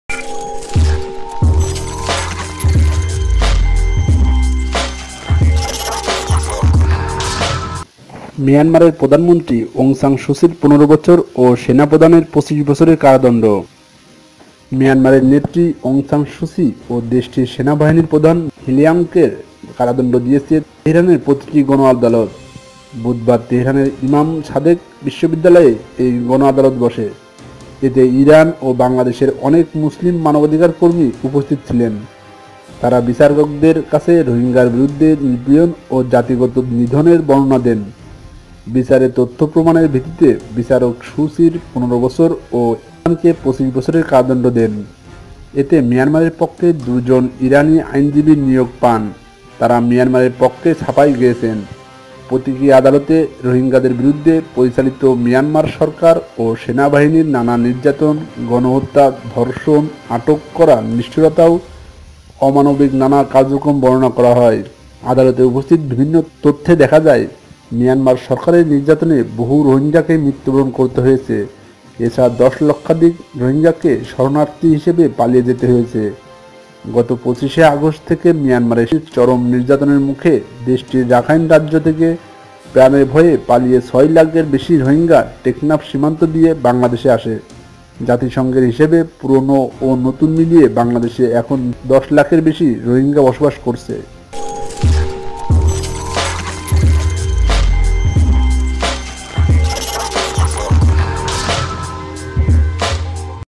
রোহিঙ্গা নিপীড়নে সু চির ১৫ ও সেনাপ্রধানের ২৫ বছর কারাদণ্ড।(ভিডিও)
রোহিঙ্গা নিপীড়নে সু চির ১৫ ও সেনাপ্রধানের ২৫ বছর কারাদণ্ড ।
ইরানে রোহিঙ্গা নিপীড়ন , সু চির ১৫ বছরের কারাদণ্ড